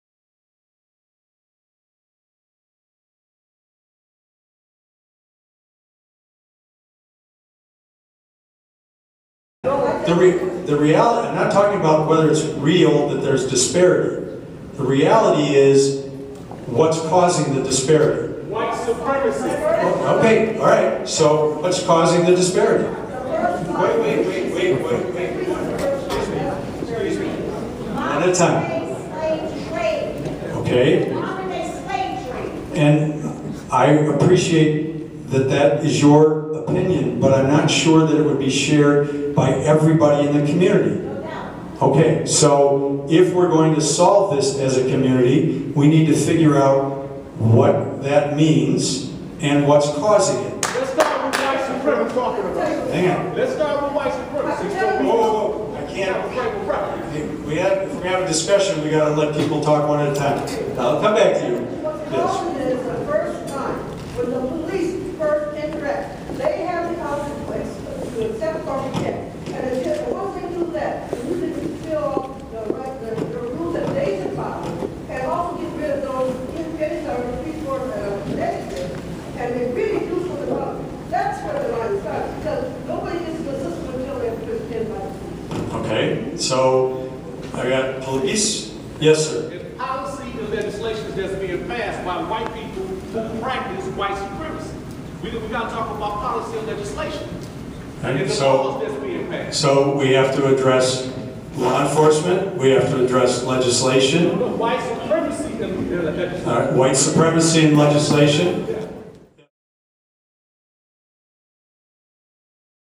Milwaukee Residents Blame Racism for City Problems
At a community forum about crime and community, several Milwaukee residents declared that racism is to blame for the problems the city faces.